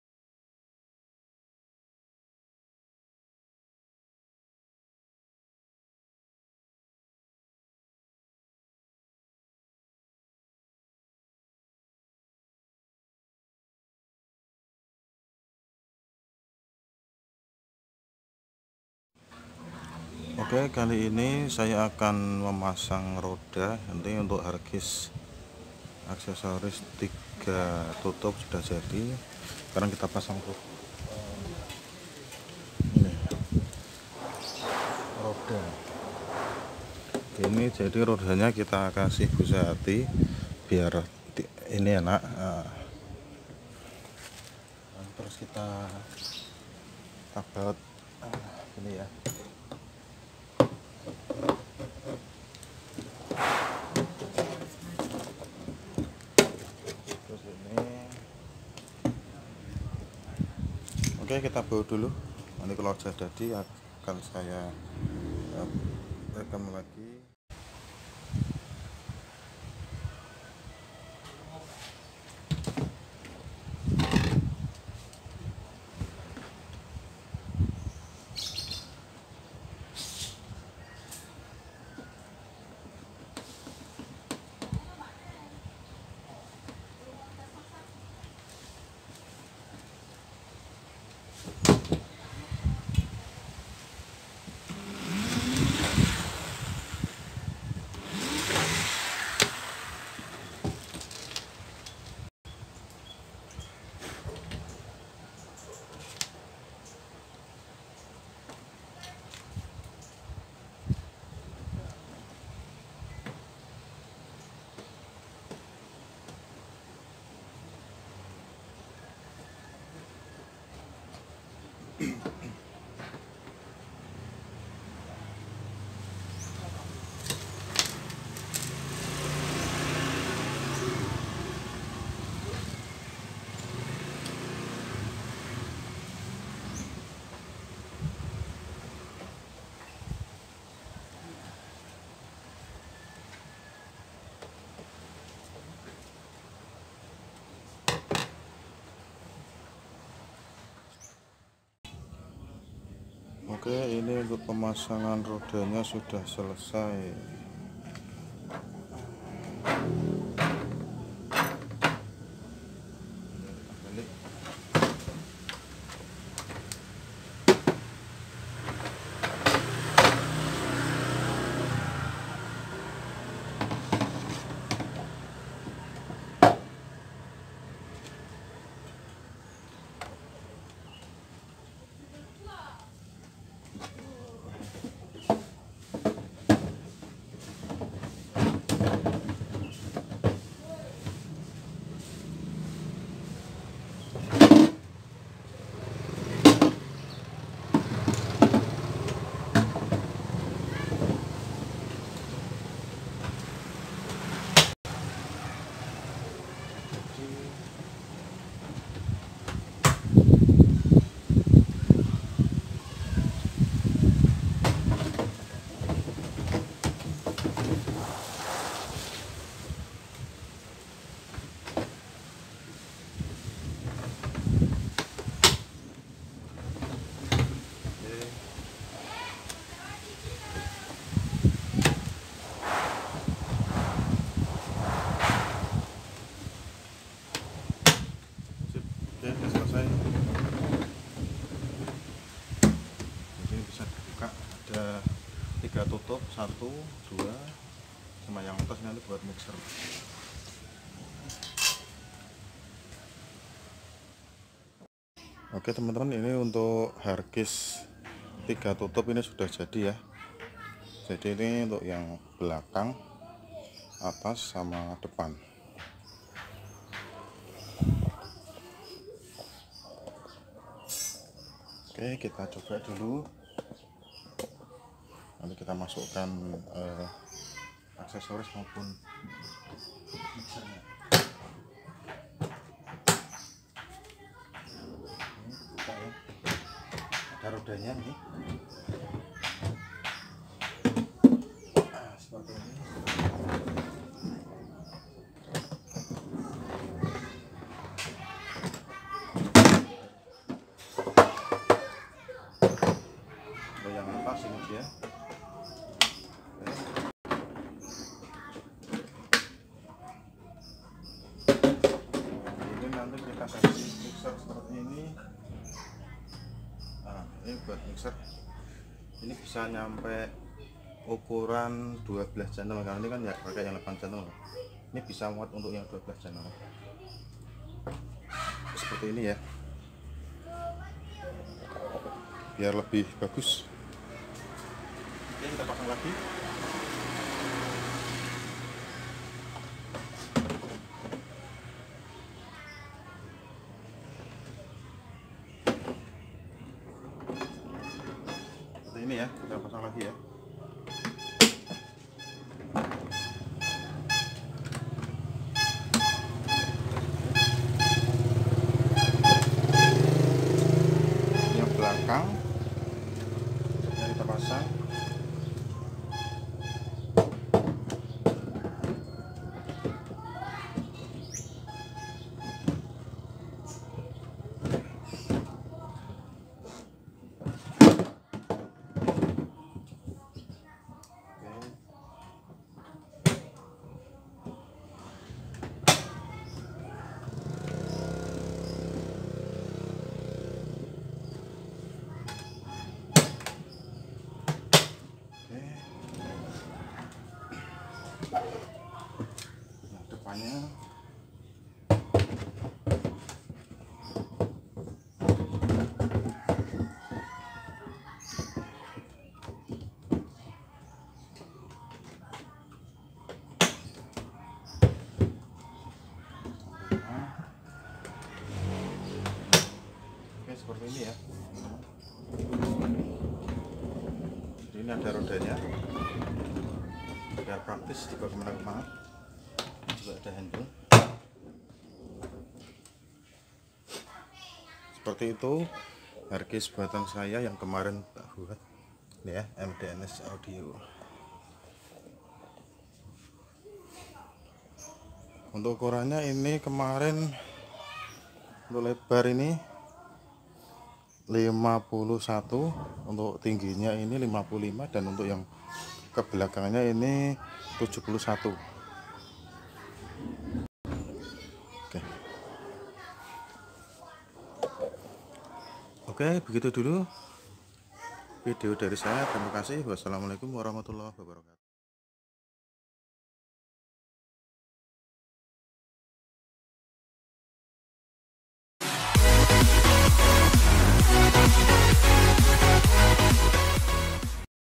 membuat sekaligus Review hardcase aksesoris sound system 3 tutup
pesanan hardcase aksesoris buat power,rqualiser+mixer.pake 3 tutup jadi satu dengan mixer